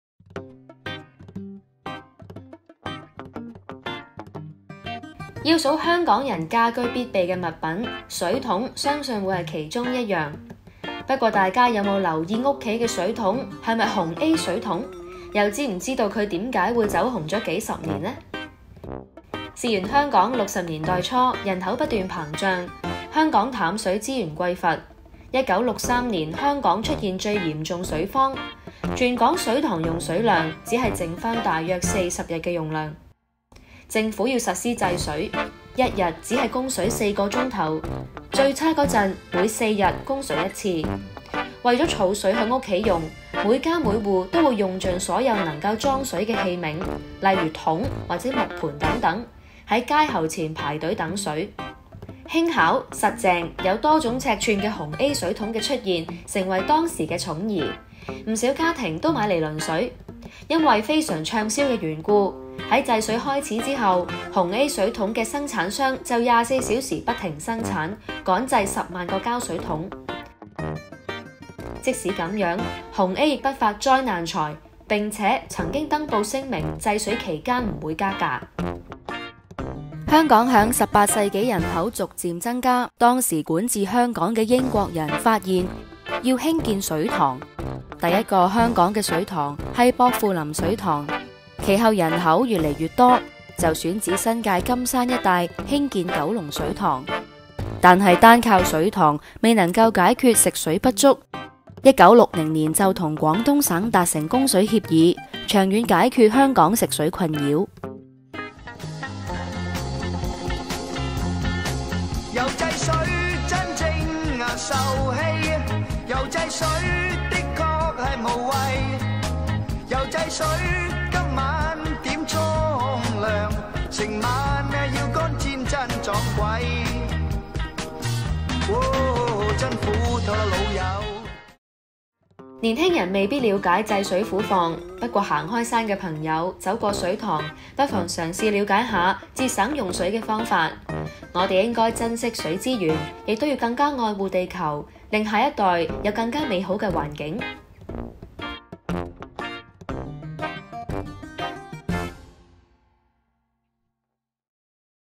【香港瑰寶 紅A水桶】30.12.2020
影片來自團結香港基金
Our Hong Kong Foundation 團結香港基金

【老牌盛載 遺忘歷史】

要數香港人家居必備物品，水桶相信會是其中一樣。不過大家有否留意家裡的水桶是否「紅A水桶」，又知不知道它，為甚麼走紅數十年?

事源香港60年代初，人口不斷膨脹，香港淡水資源匱乏，1963年香港就出現最嚴重水荒，全港水塘用水量只餘約40日，政府要實施制水，一日只供水4小時，最差的時候每4天供水一次。為儲水在家儲用，每家每户會用盡所有夠能載水的器皿，桶、木盆等等排在街喉前等水。

輕巧、實淨、有多種尺寸的「紅A水桶」的出現，成為當時寵兒，不少家庭都買來輪水。而因為十分暢銷的原故，自制水開始後紅A水桶的生產商便24小時不停生產，趕製十萬個膠水桶。但即便如此，紅A亦決不發「災難財」，並且曾登報聲明制水期間不會加價。

香港在18世紀人口逐漸增加，當時管治香港的英國人發現要興建水塘，第一個香港的水塘是薄扶林水塘，其後人口逾來逾多，就選址新界金山一帶興建九龍水塘，但單靠水塘未能解決食水不足，1960年便與廣東省達成供水協議，長遠解決香港食水困擾。

年輕人未必了解制水苦況，不過行開山的朋友，走過水塘不妨嘗試了解下節省用水的方法，始終水資源十分珍惜，我們亦要更愛護地球，能令下一代有更美好的環境。